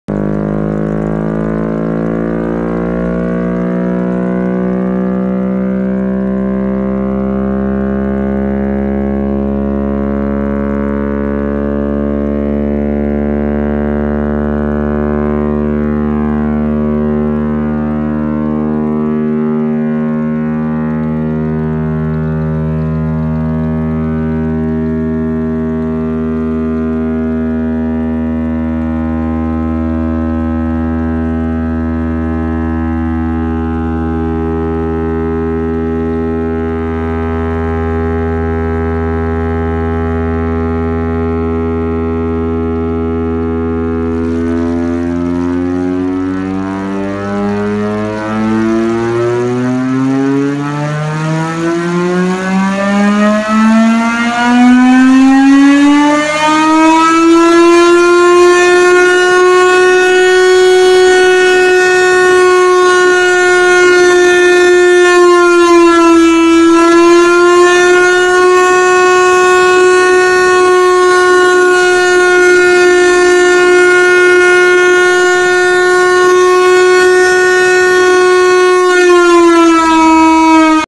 11/4/18 / Chrysler Air Raid Siren at Ederville
CARTHAGE - Vintage Chrysler air raid siren (repowered with replacement motor) in collection of Ken Eder, demonstrated at annual Ederville festival. Mounted on a 1969 Ford/John Bean fire engine that served Snow Camp, NC.